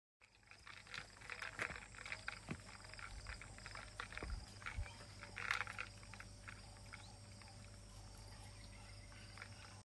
Honey Badger scurries through lodge
What a lucky sighting, two honey badgers went quickly under the deck/patio at the lodge and it was lucky we got this quick video of one! While these animals are fairly common, we don't often get to see them as they are usually shy. That said, you also don't want to get into a fight with one!
Video: Cole van Wijk